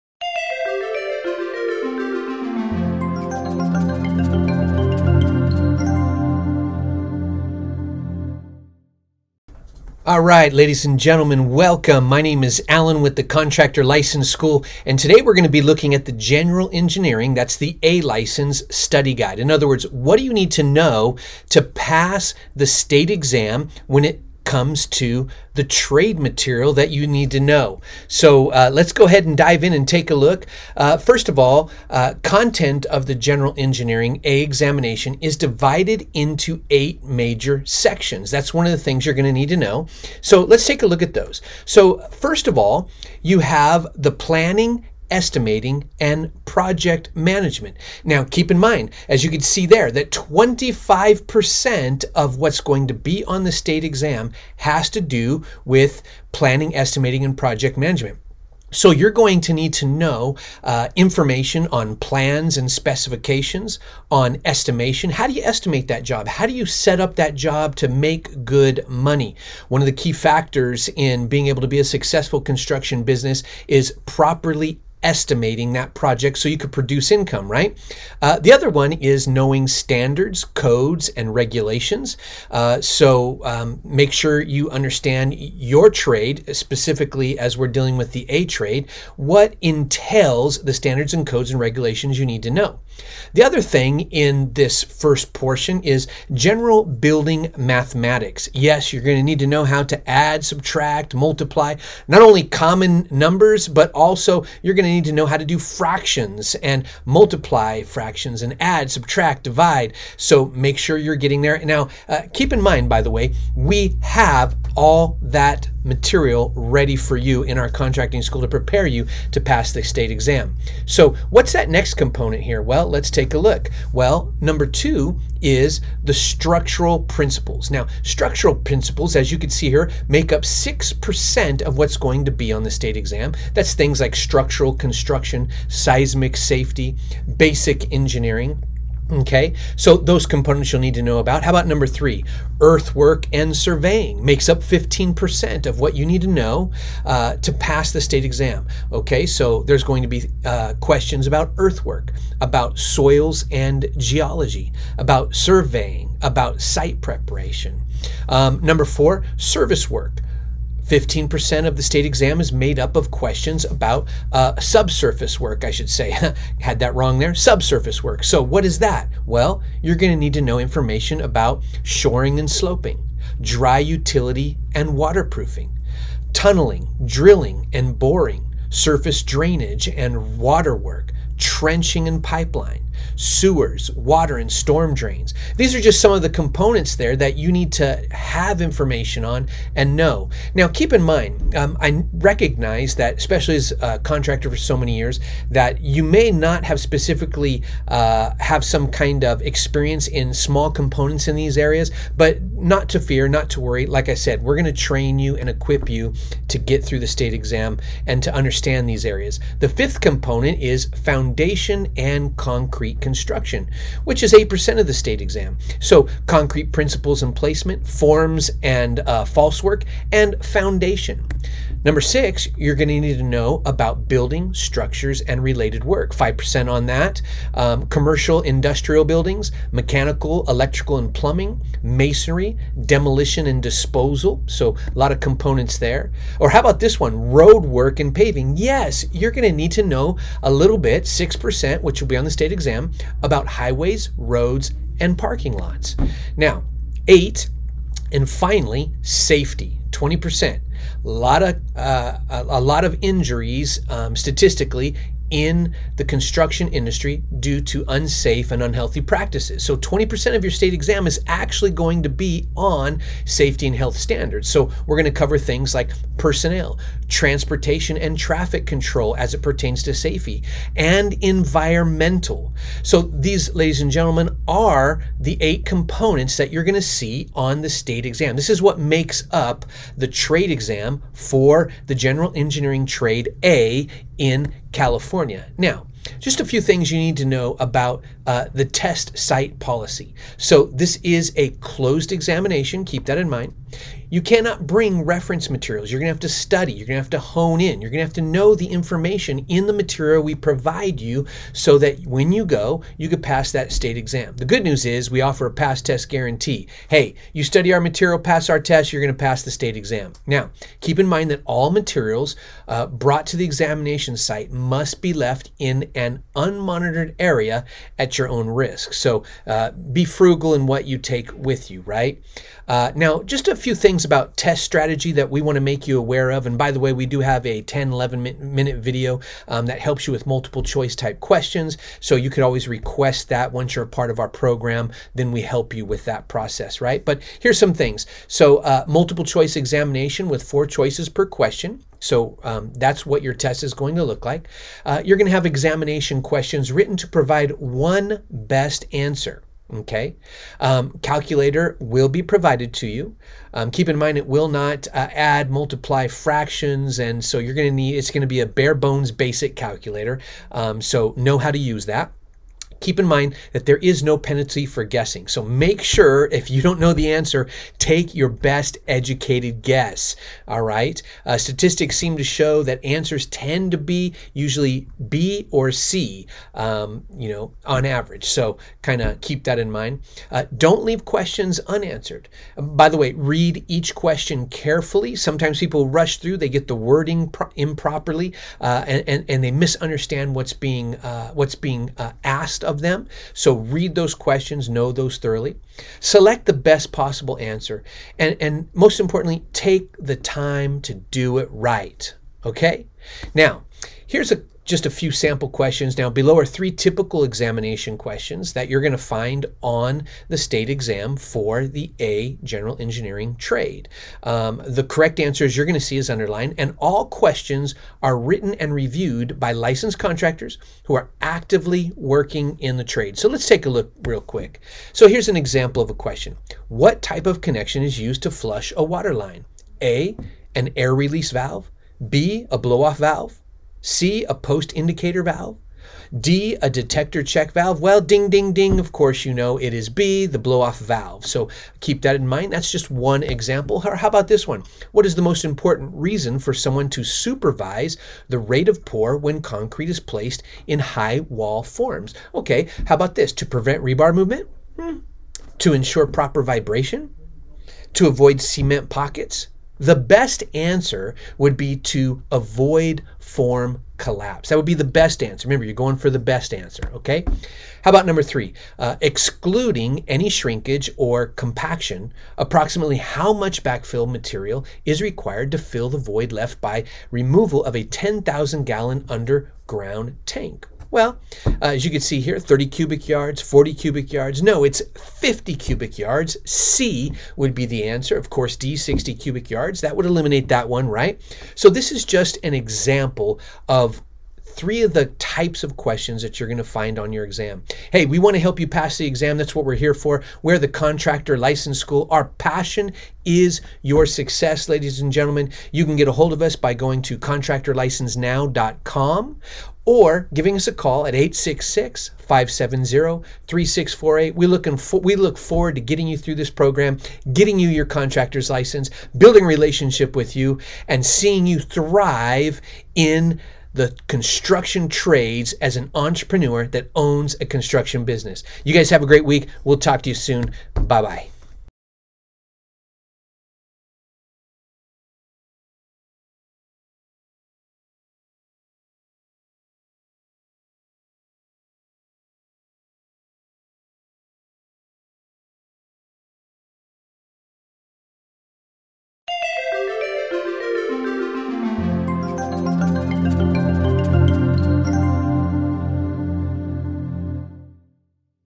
What is on the General Engineering Contractor exam? [General A License Examination Guide]
This is the General Engineering Contractor Trade Guide for the A trade license in California.

What kind of questions are on the general A construction trade exam?  In this video, we cover the 5 major segments of the General contractor state exam for California, we also give you the secret strategy for passing your exam,  and finally, this General A examination guide tutorial will give you a sample of actual questions found on the CSLB exam.  In this video, we go over the five major segments that make up the exam as seen below:

See you in our classroom or in our LiveStream training.